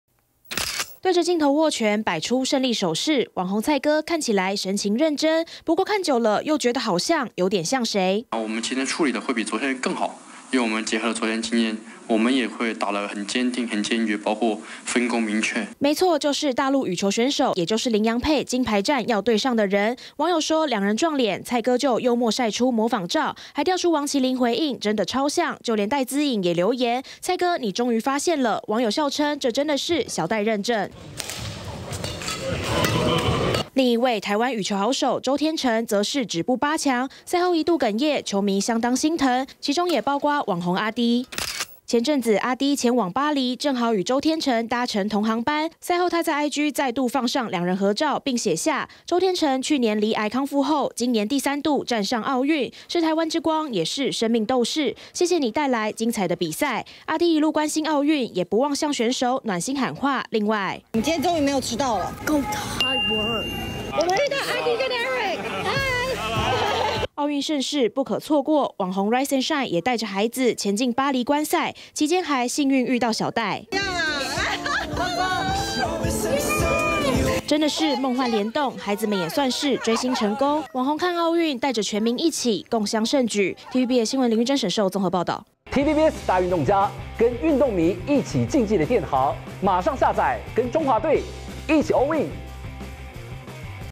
麟洋配金牌戰! 蔡哥神模仿「大陸選手」釣出王齊麟.小戴回應｜TVBS新聞
支持台灣選手，全民一起瘋奧運，網紅阿滴、Rice＆Shine一家人，更是直接飛到巴黎，現場觀賽。而網紅蔡哥卻是用不一樣的方式「應援」，因為被說長的像大陸羽球選手梁偉鏗 ，他就曬出模仿照，擺出相同的表情動作，還釣出王齊麟、戴資穎回應。
台灣 奧運 阿滴 Rice＆Shine#蔡哥#王齊麟#戴資穎